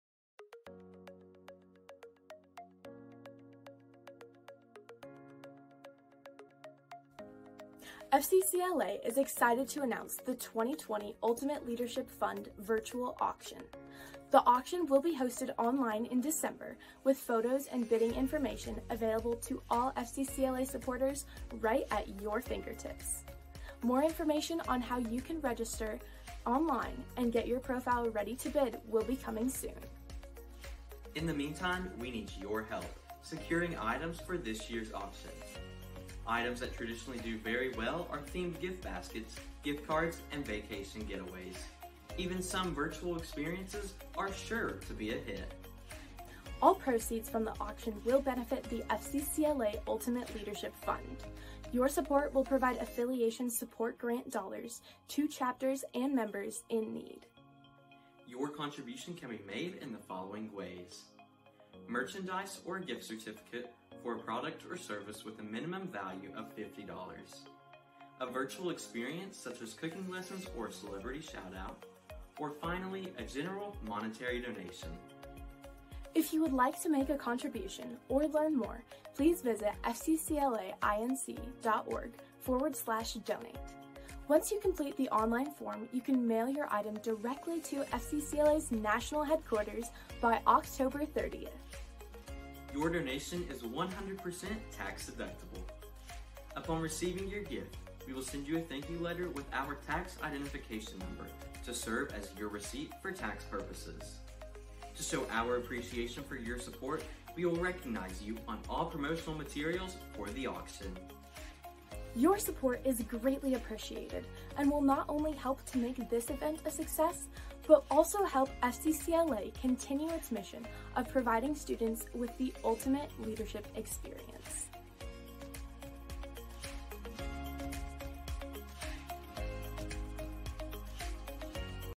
FCCLA's Ultimate Leadership Fund - Virtual Auction
FCCLA is excited to announce the 2020 Ultimate Leadership Fund Virtual Auction. The auction will be hosted online in December with photos and bidding information available to all FCCLA supporters right at your fingertips. More information on how you can register online and get your profile ready to bid will be coming soon.

In the meantime, we need your help to secure items to feature in this year’s auction! Items that traditionally do very well in an auction are themed gift baskets, gift cards, and vacation stays. Even some creative ‘virtual experiences’ are sure to be a hit! All proceeds from the auction benefit the FCCLA Ultimate Leadership Fund. Your support will provide funds to support students who need your help to be able to participate in FCCLA this year.